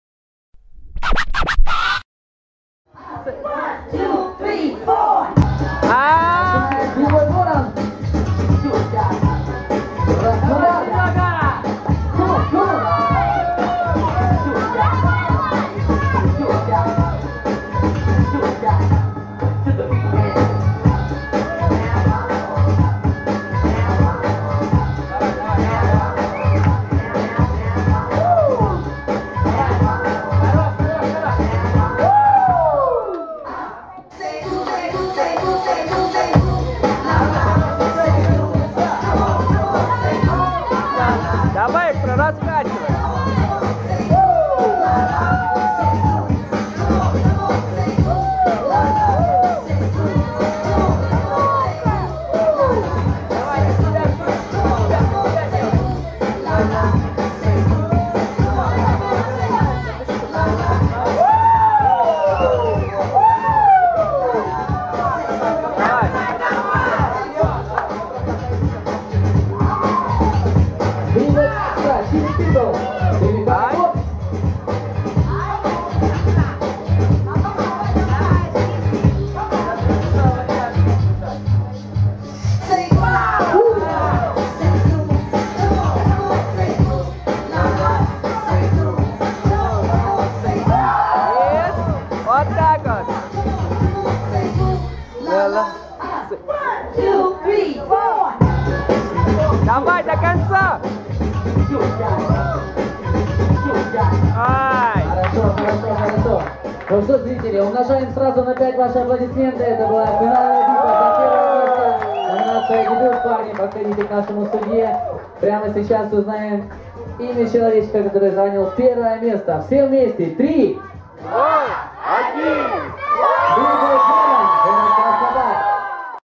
Воронов Сергей vs Extro - финал - FIGHT BY DANCE 9
Break Dance 1*1 «Дебют» (до 12 лет, с опытом танца до 1 года)
Победа: Воронов Сергей
22 мая 2016г.
г. Ростов-на-Дону